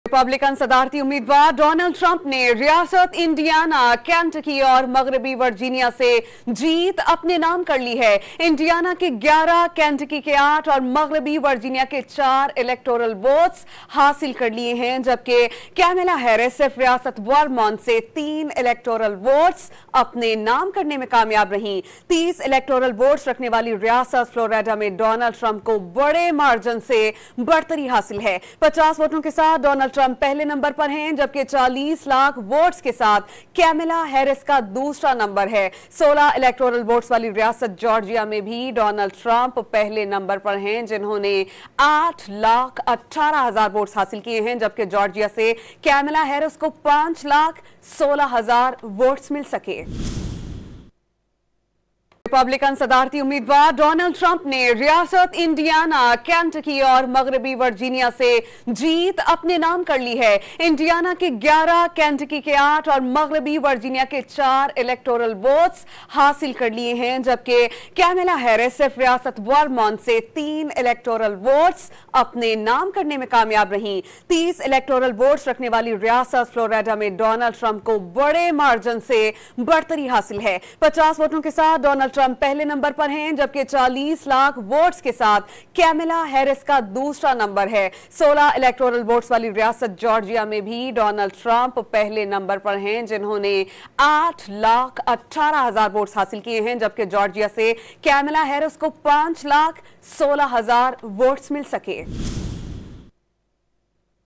US Election 2024: Kamala Harris Victorious In Vermont | Breaking News | Dawn News
- امریکی انتخابات 2024: کملا ہیرس ورمونٹ میں کامیاب